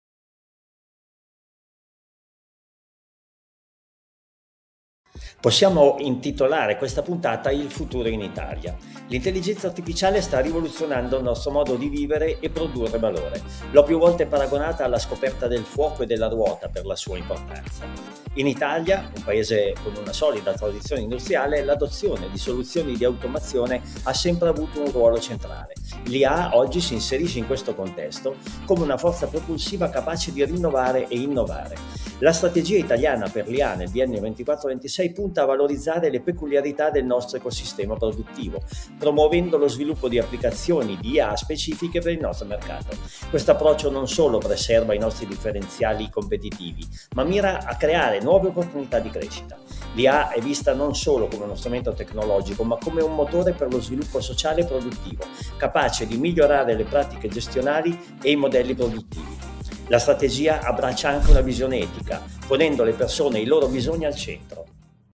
💊 "Pillola del giorno" di Giroidea! - AGID Futuro
🇮🇹 Il Futuro in Italia: Scopri come l'Intelligenza Artificiale sta riscrivendo le regole!

- L'IA è una rivoluzione paragonabile al fuoco e alla ruota. In Italia, sta trasformando il nostro panorama industriale. ⚙️🔥

- Con la strategia IA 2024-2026, puntiamo a soluzioni uniche per il nostro mercato, preservando la nostra identità e creando nuove opportunità di crescita. 📈🌿

- Non solo tecnologia, ma un motore per lo sviluppo sociale e produttivo, con un approccio etico che mette le persone al centro. 👥❤️

Unisciti a noi in questo viaggio di innovazione e scopri come l'Italia è pronta a brillare nel panorama globale. 🌍

Contenuto auto prodotto con diritto di critica.